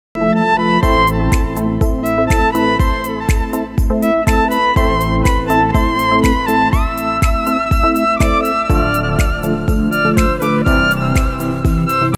From where to get scooters on Rent in Goa!?
The Most Important Question for all Tourists, isn't it? Where do we get the scooters from!

Well, here is a short reel to answer your question!

Credits- @goa_darling ✅Save and share for future references

🗣You can hire a scooter or bike anywhere in north goa

✅ Calangute
✅ Baga
✅Anjuna
✅candolium
✅Mapusa
✅Panjim

🗣Prices

Facino  350 Rs
Activa 300 Rs
Bullet 600-750 Rs
Avenger 500 Rs

Save , share and feel the vibes of goa 💕